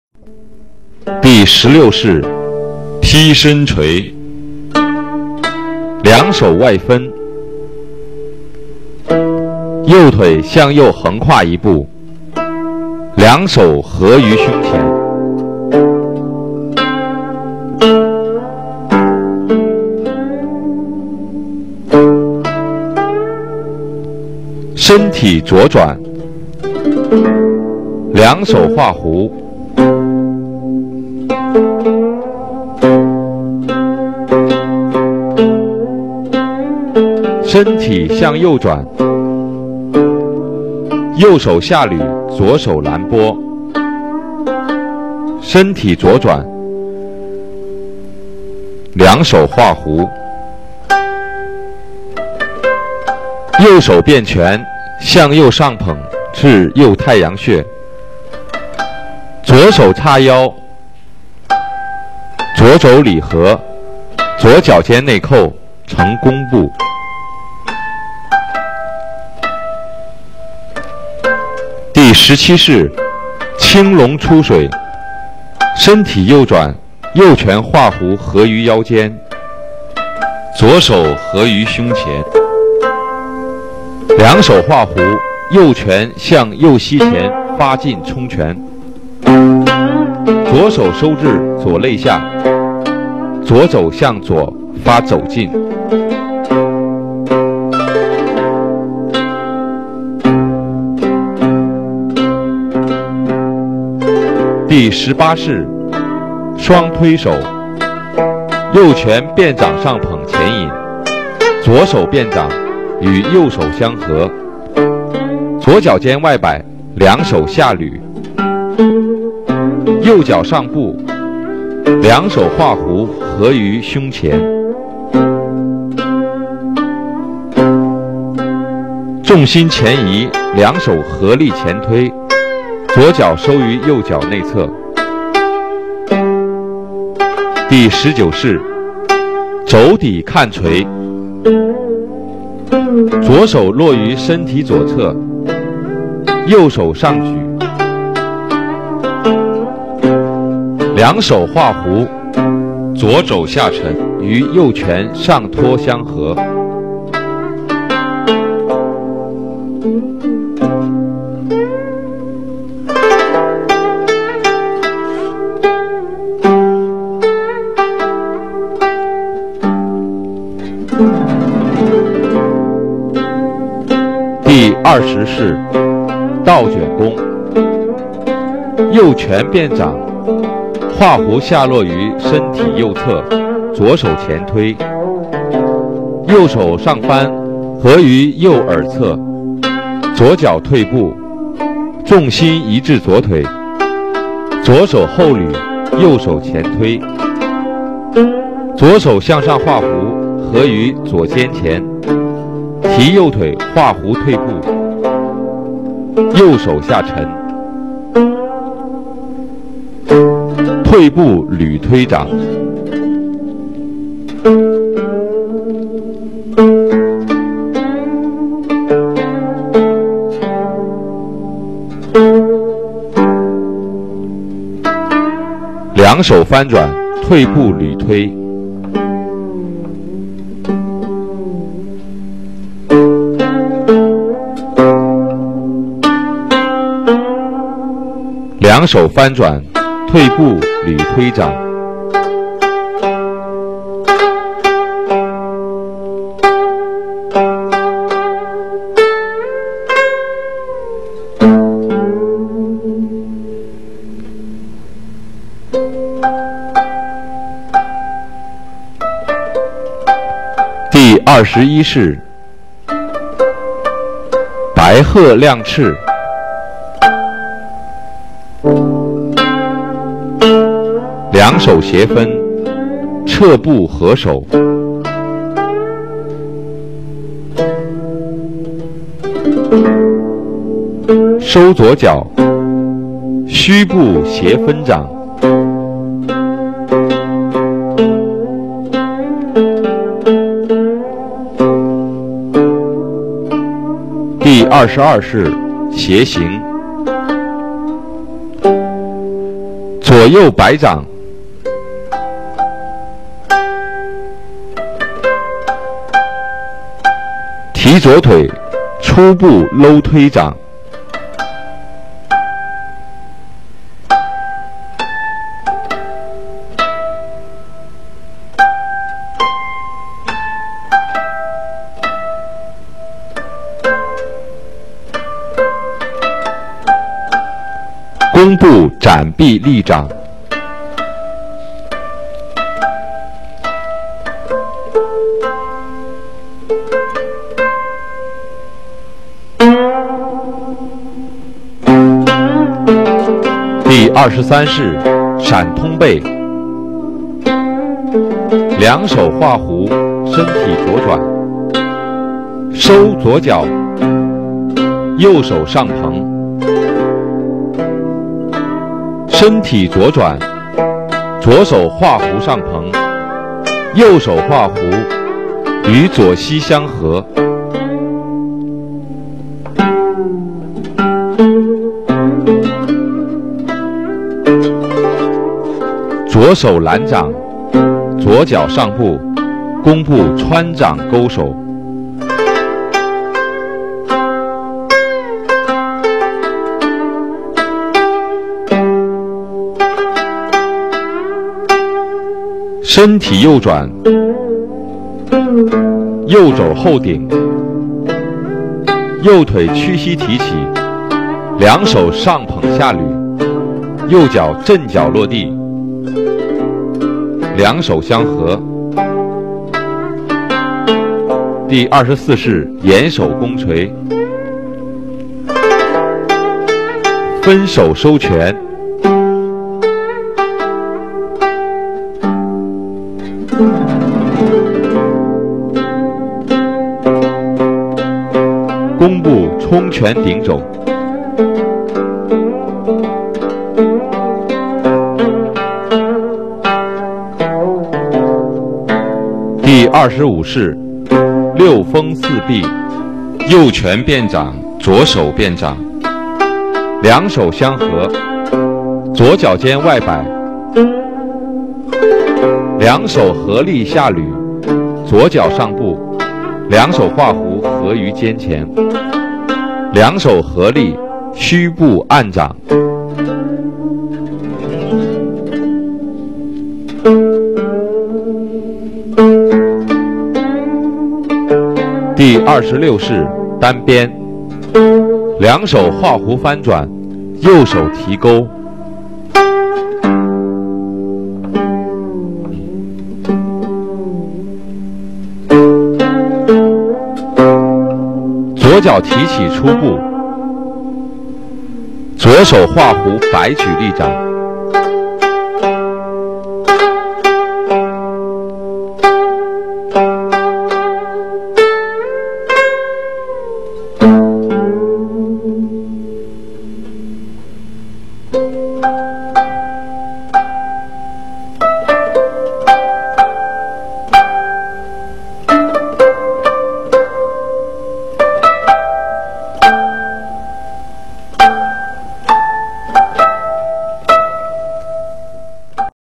Chen style Tai Chi Old Frame 1st routine tutorial 16~26
陈式太极拳老架一路16~26式 教学版 王二平